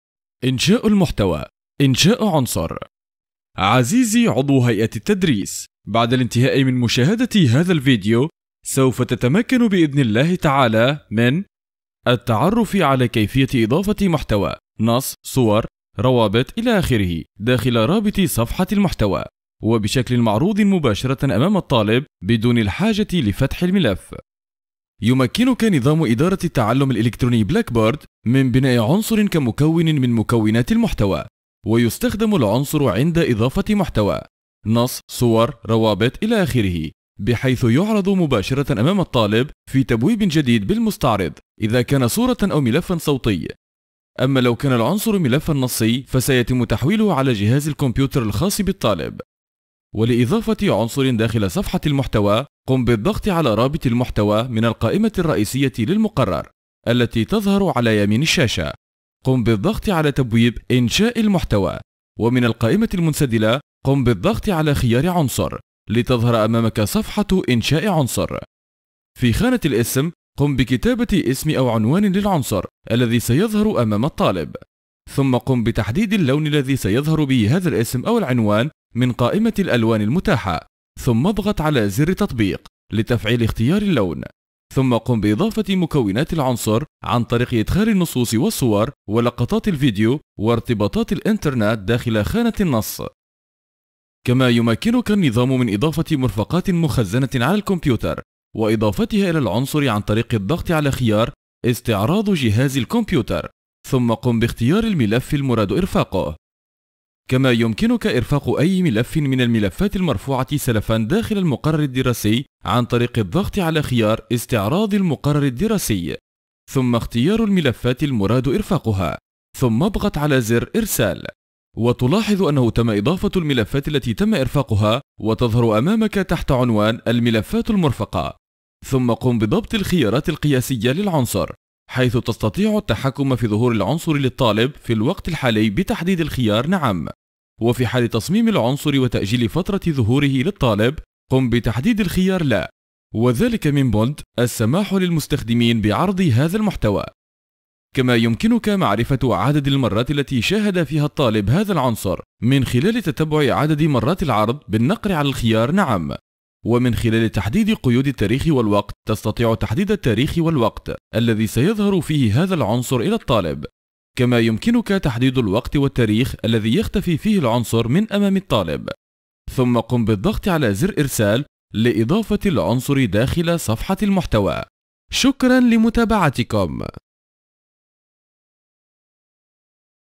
إنشاء المحتوى إنشاء عنصر -المستوى الأساسي- أعضاء هيئة التدريس
عزيزي عضو هيئة التدريس بعد الإنتهاء من مشاهدة هذا الفيديو سوف تتمكن بإن الله تعالى من :
- التعرف على كيفية إضافةمحتوى نص صورة روابط ..الخ